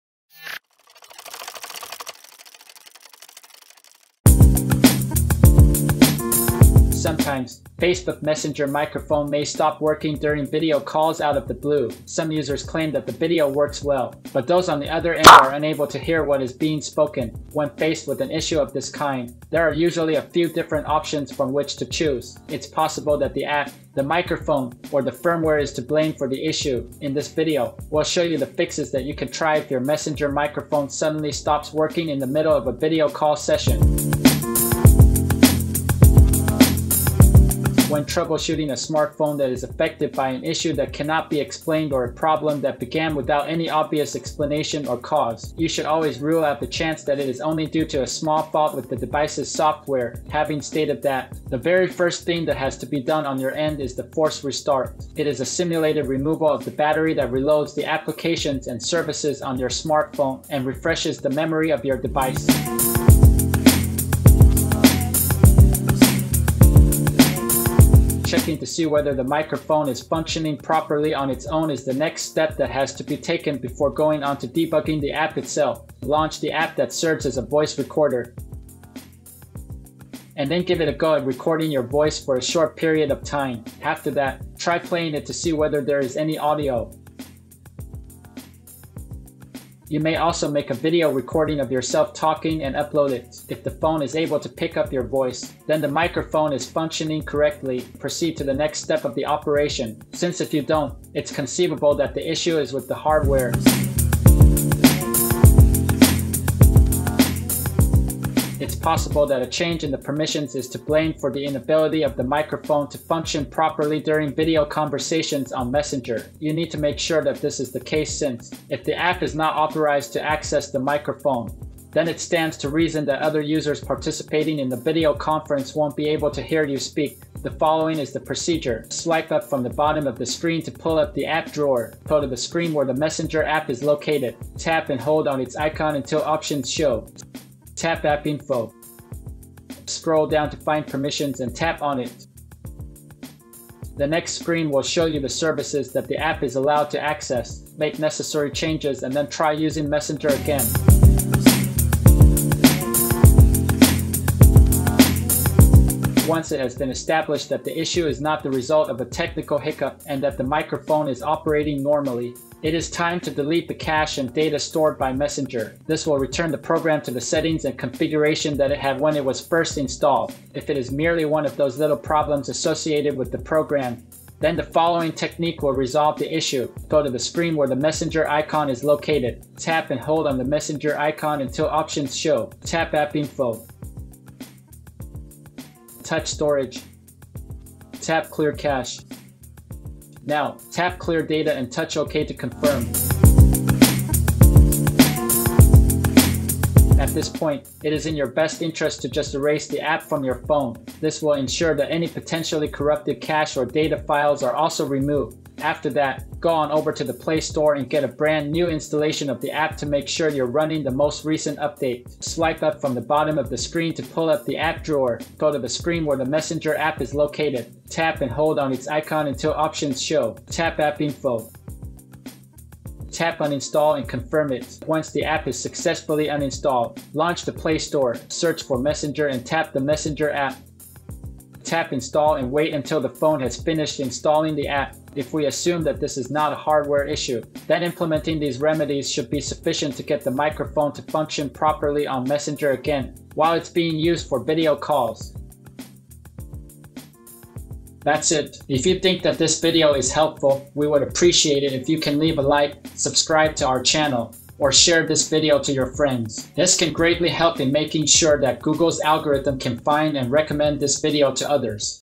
SOLVED: Facebook Messenger Mic Not Working On Video Calls
Sometimes, Facebook Messenger microphone may stop working during video calls out of the blue. Some users claim that the video works well, but those on the other end are unable to hear what is being spoken. When faced with an issue of this kind, there are usually a few different options from which to choose. It's possible that the app, the microphone, or the firmware is to blame for the issue.
In this video, we’ll show you the fixes that you can try if your Messenger microphone suddenly stops working in the middle of a video call session.